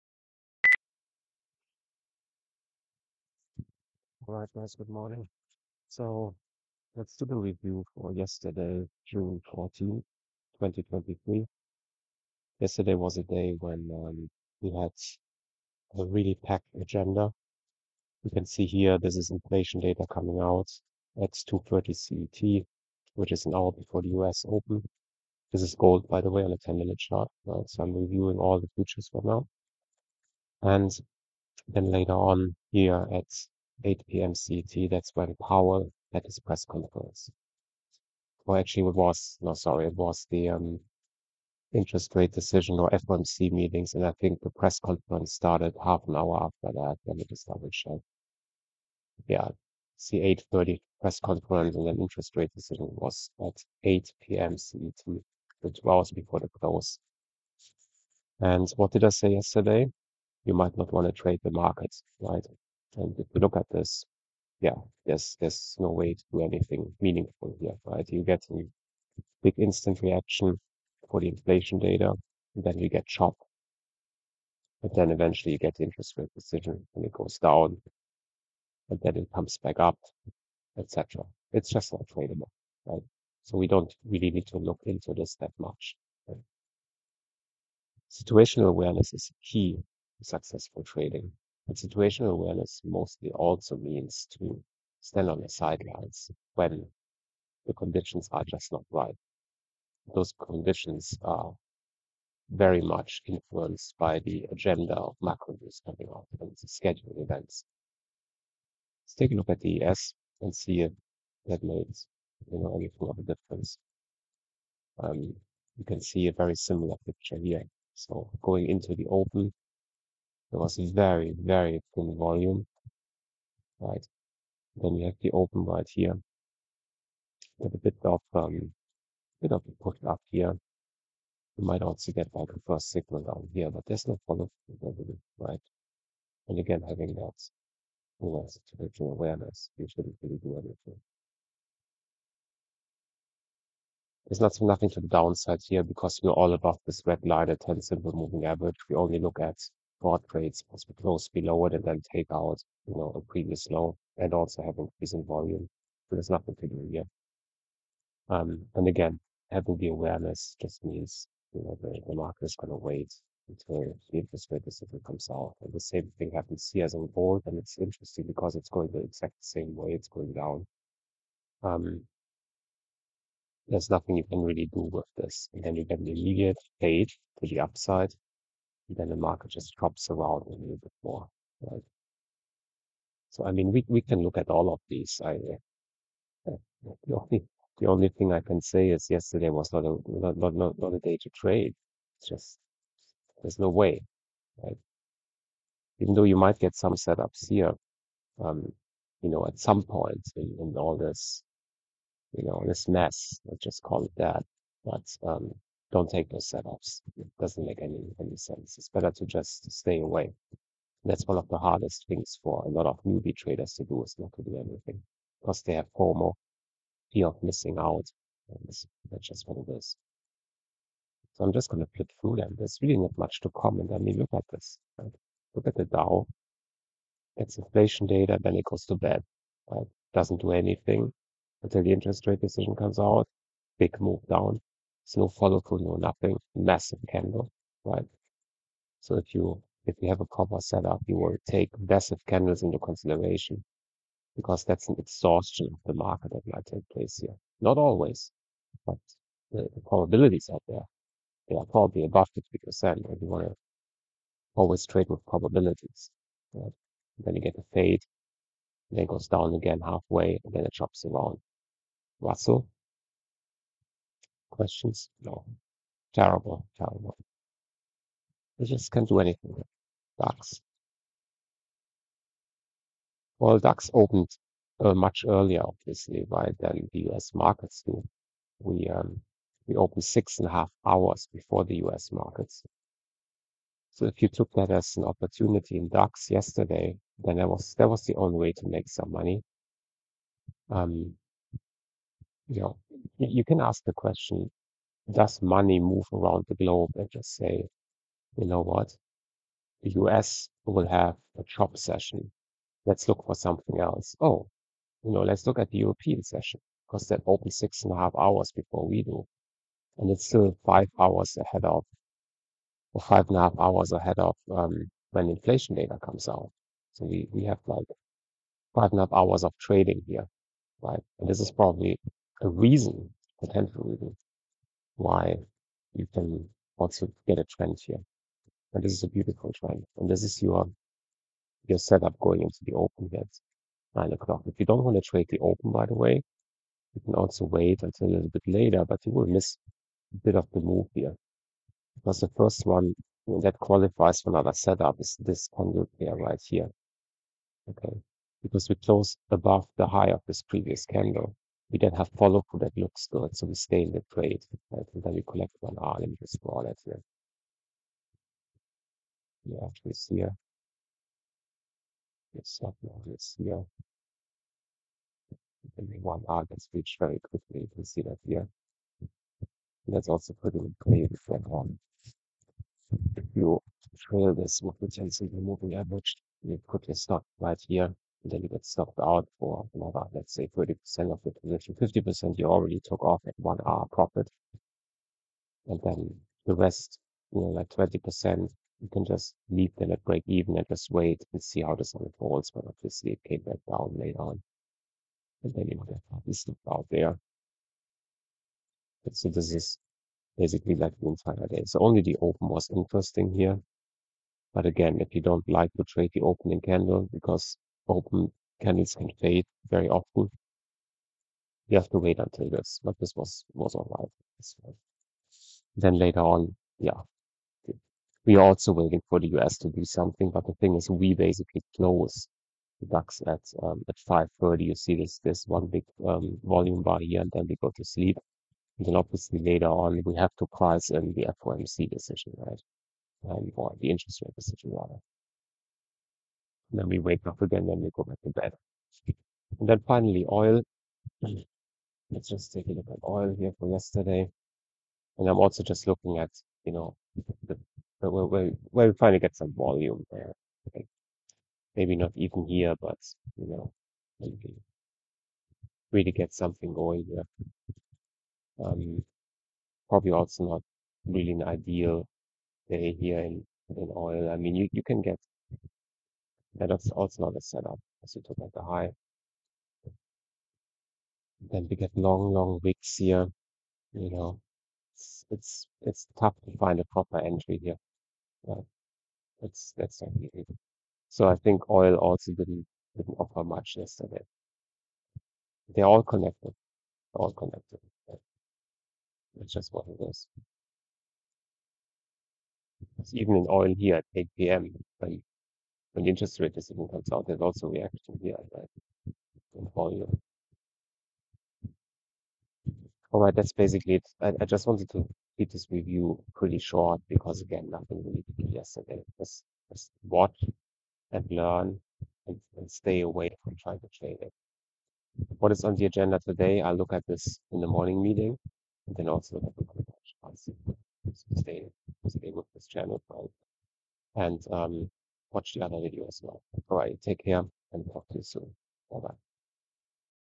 Rightfully staying on the sidelines - Review of trading opportunities for June 14, 2023
In these review videos, I look back at trading opportunities based on the levels of interest that identified in the morning meeting recordings. This gives you a real insight into how to trade low timeframe setups, so that you can maximize trading size while keeping your risk exposure low.

Note: none of these videos or comments in them or about them is financial advice.